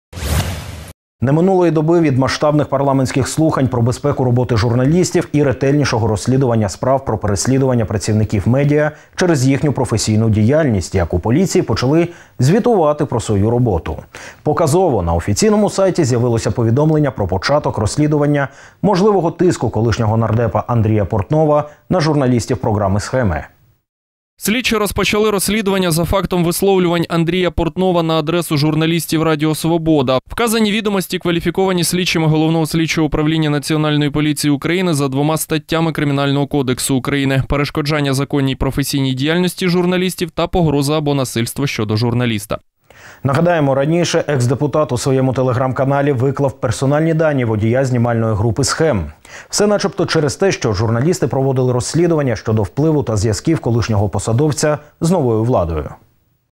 Поліція розпочала розслідування можливого тиску Андрія Портнова на журналістів
Не минуло і доби від масштабних парламентських слухань про безпеку роботи журналістів і ретельнішого розслідування справ про переслідування працівників медіа через їхню роботу, як у поліції почали звітувати про роботу. Показово!